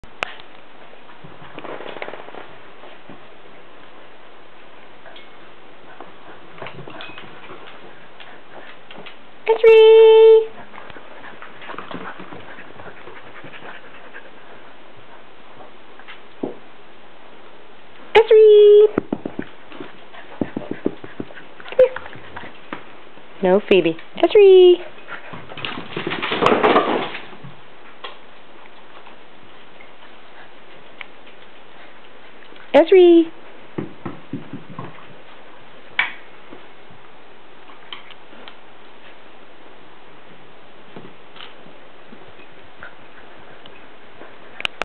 Ezri Discovers Tennis Balls
Baby Ezri has discovered tennis balls!